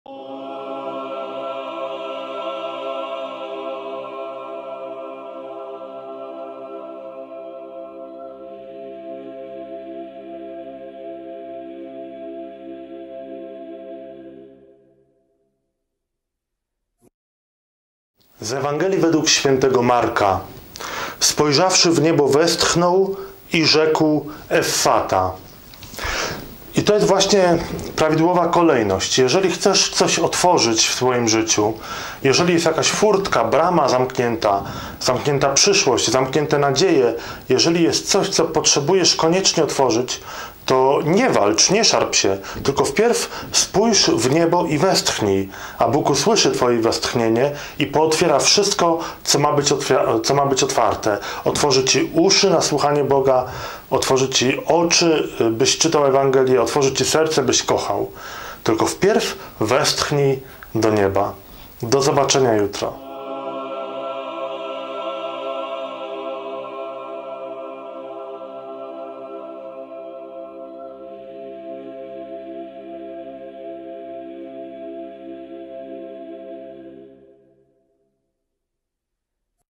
Kilka słów o Słowie 13 II 2015
Mój komentarz do Ewangelii na piątek, 13 lutego 2015
Zachęcam, by przeczytać najpierw fragment Ewangelii Mk 7,31-37